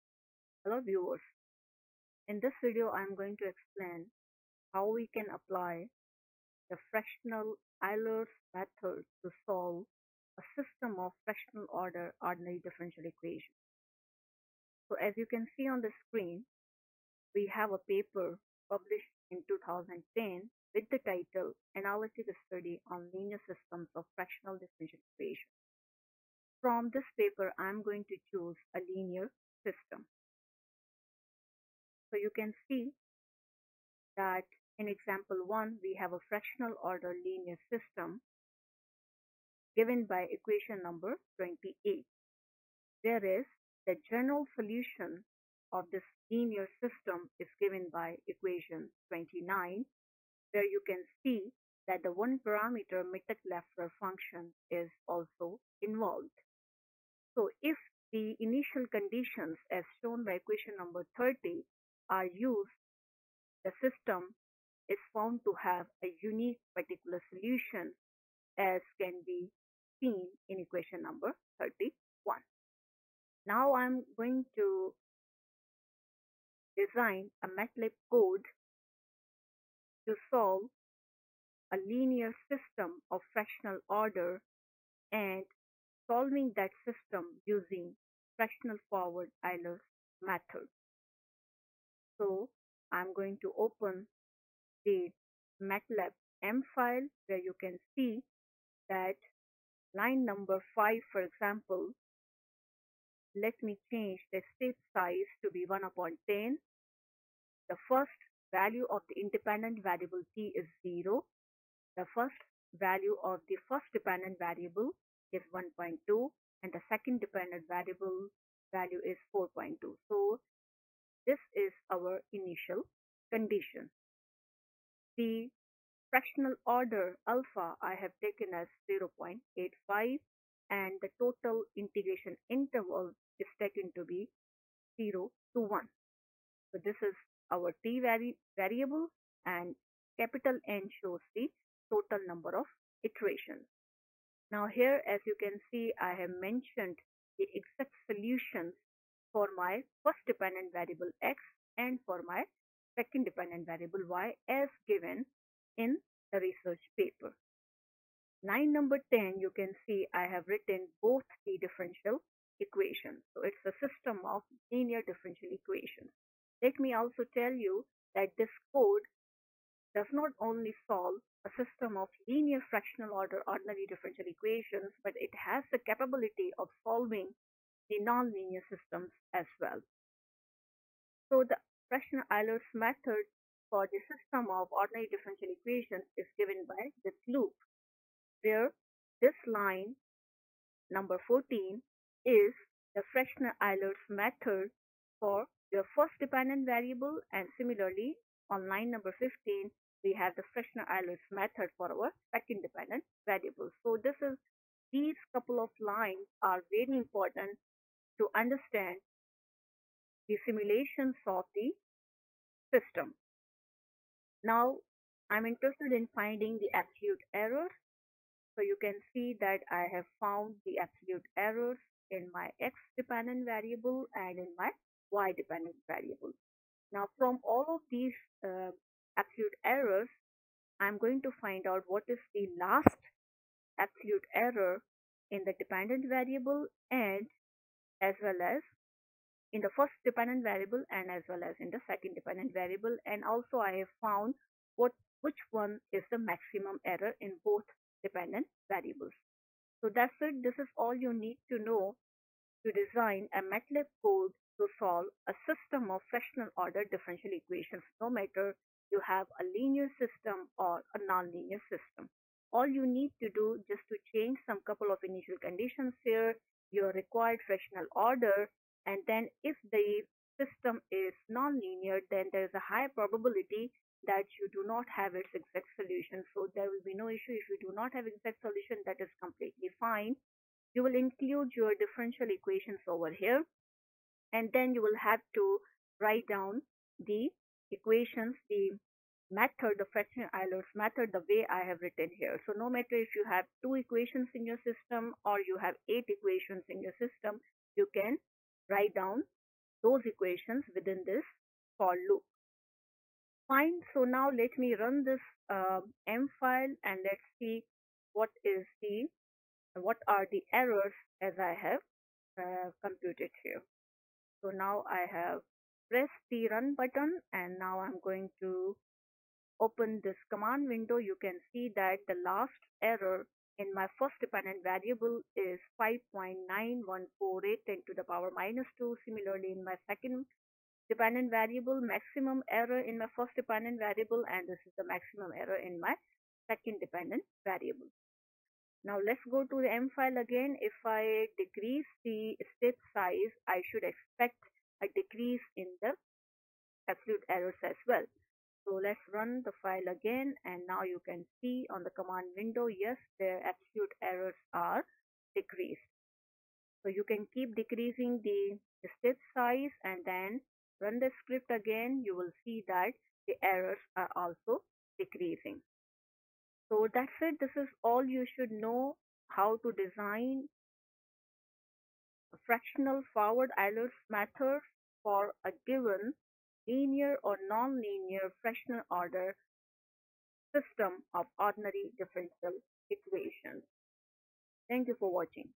Lecture 01: 2 of 2 MATLAB code for Fractional Forward Euler's Method for a System of ODEs
This lecture belongs to the field of Fractional Calculus. In this lecture, a MATLAB Code of the Fractional Forward Euler's Method (Explicit version of the method) for Solving Fractional-Order System of Initial Value Problems (Linear and Nonlinear) is discussed. One can simulate a given fractional-order system defined with the Caputo differential operator.